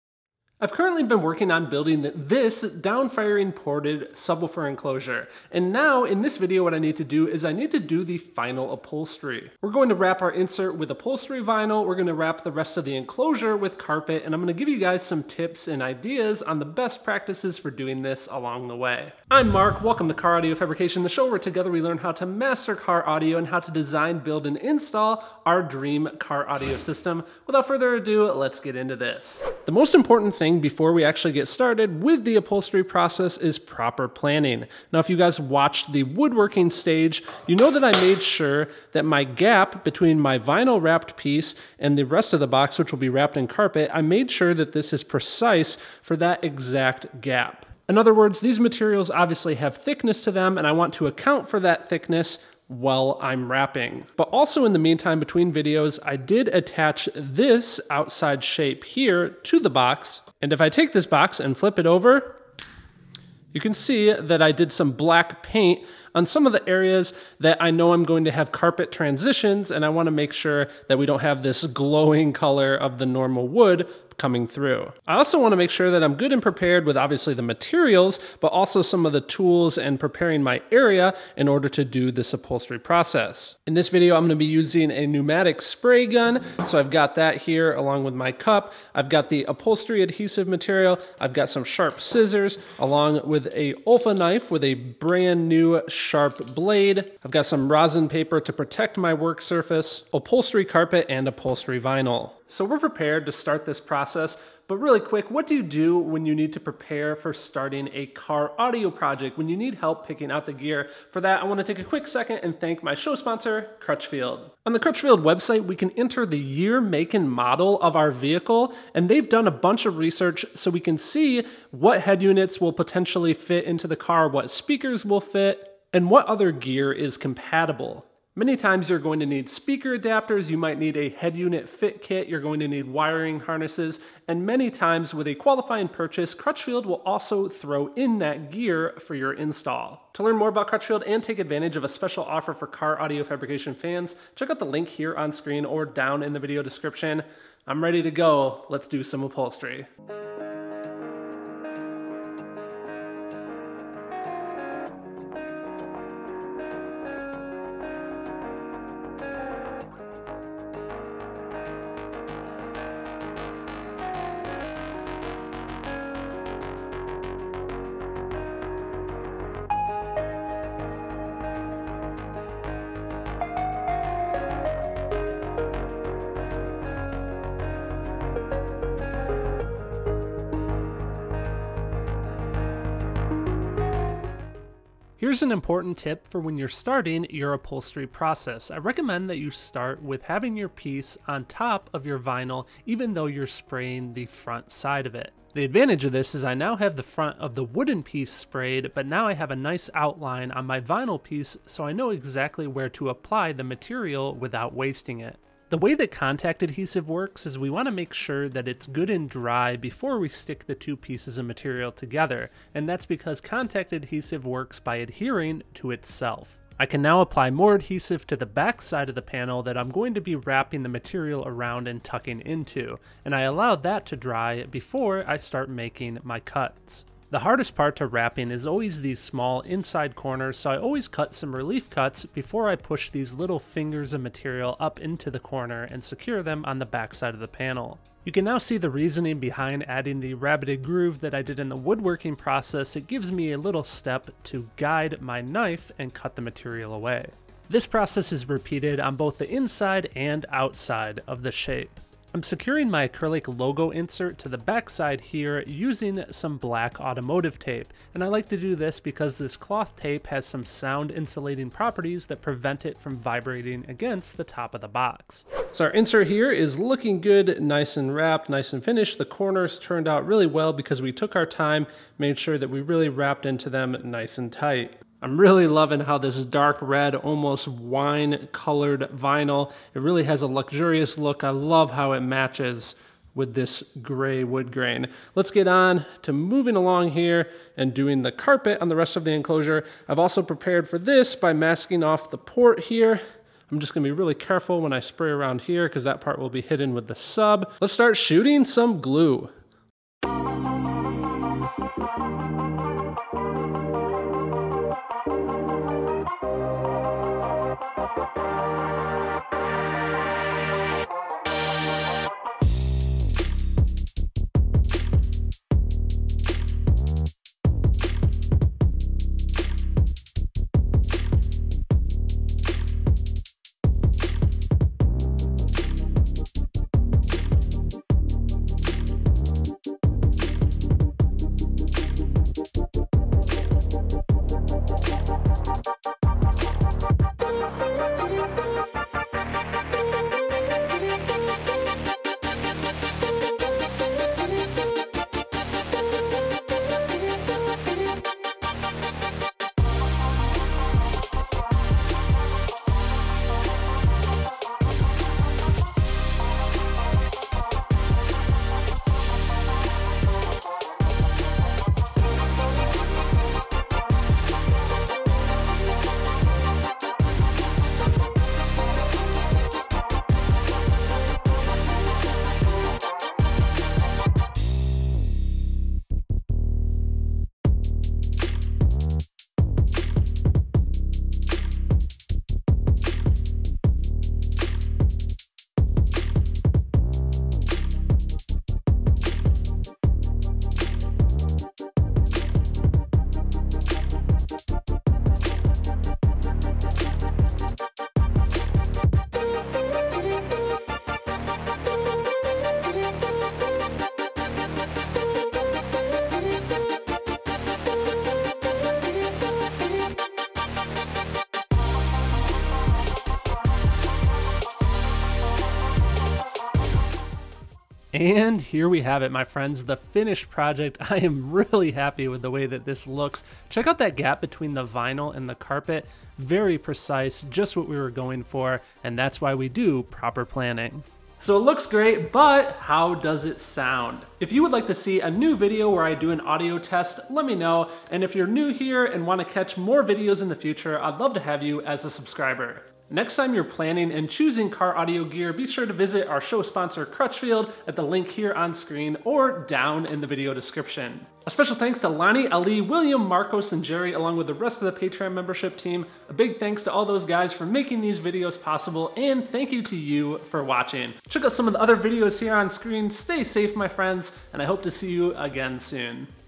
Perfect Upholstery! How to WRAP subwoofer enclosure - Carpet & Vinyl
With the woodworking complete on my custom built downfiring subwoofer box I now need to finish the upholstery process with carpet & vinyl. How can you get perfect results on making your own custom sub enclosure? Let's wrap!

✔️ New Customer?
✔️ Existing Customer?

Use of information from this video is at your own risk.